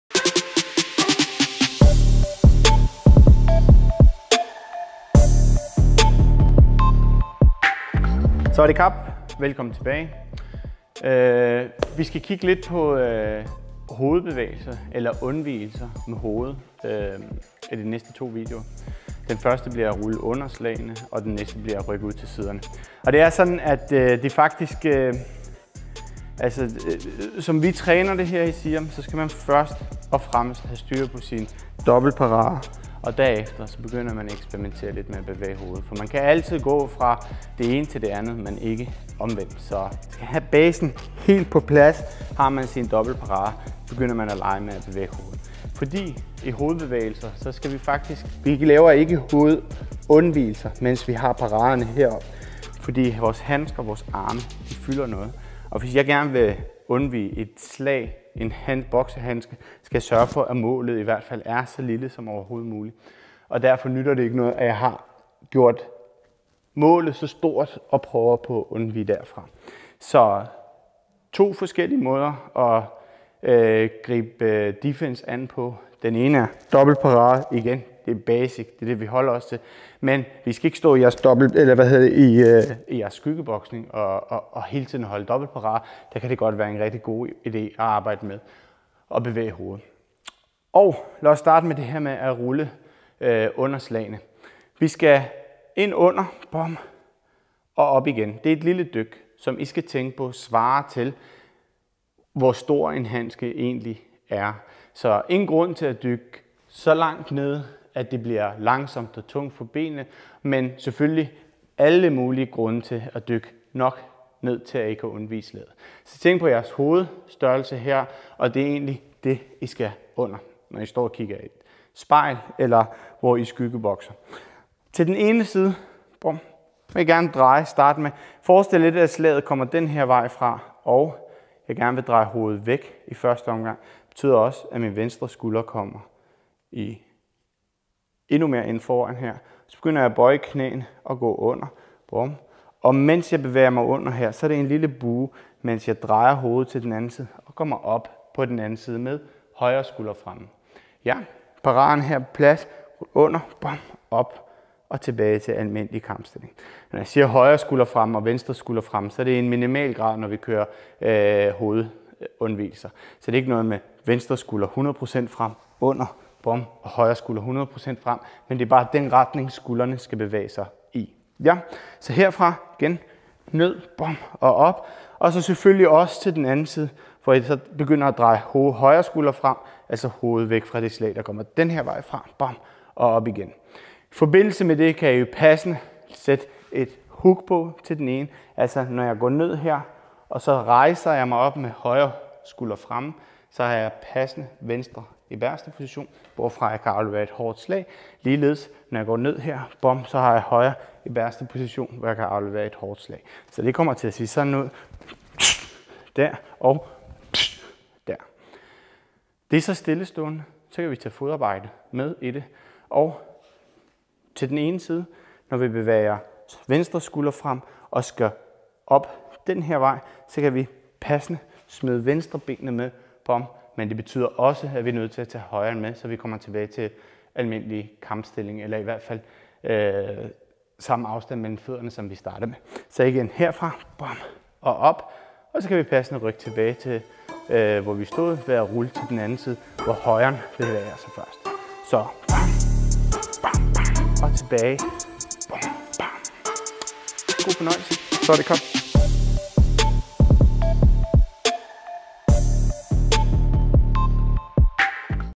Video 12 - Undvigelser pt 1
Denne video viser undvigelser i boksning. Her gennemgåes den cirkulære bevægelse hvor man ruller under slagene.